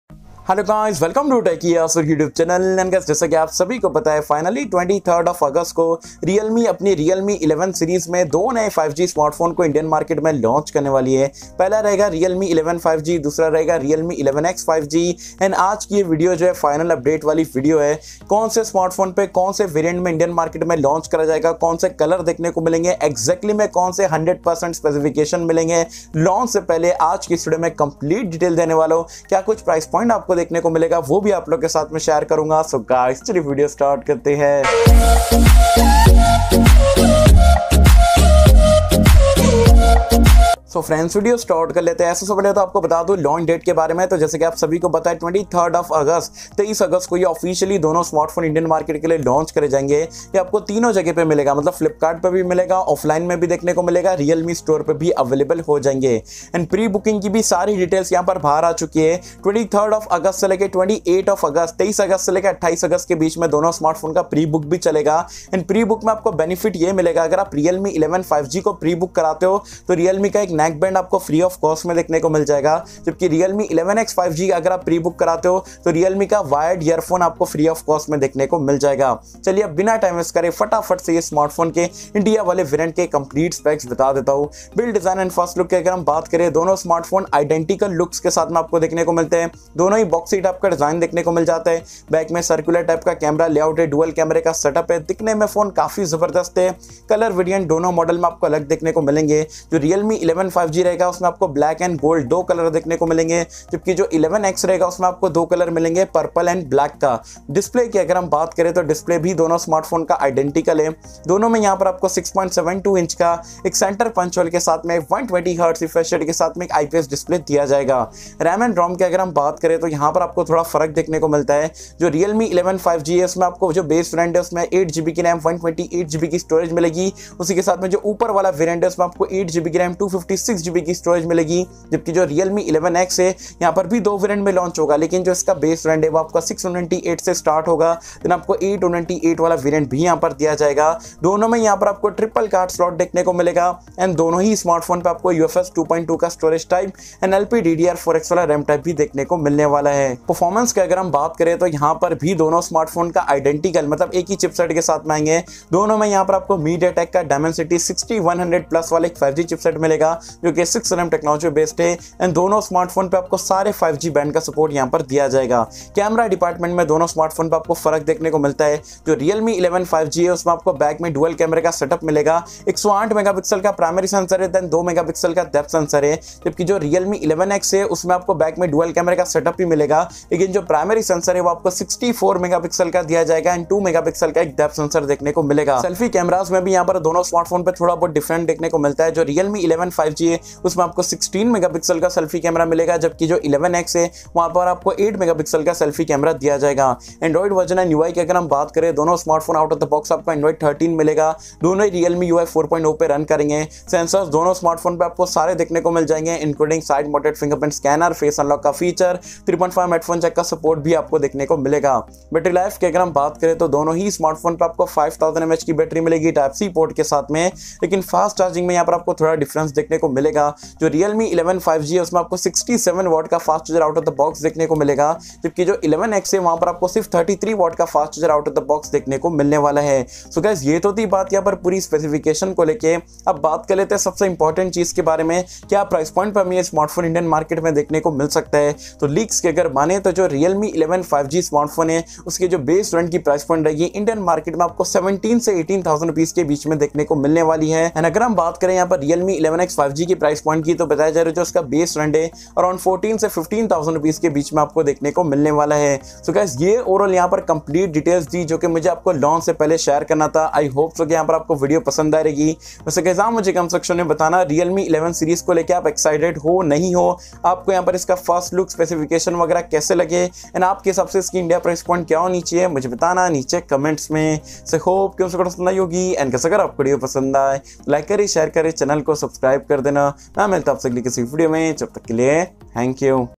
Realme 11 5G & Realme 11X 5G - Final Update 🔥 Realme 11 5G Price | Realme 11X 5G Price & Everything🔥
Hello Guys Aj Ki Iss Video Mei Meine Apko Realme 11 5g & Realme 11X 5g Smartphone Ke India Launch Ki Saari Details Diya Hu.. India Launch, Price & Specs Ki & Apne Honest Opinions Apke Sath Share Kara Hu 👍👍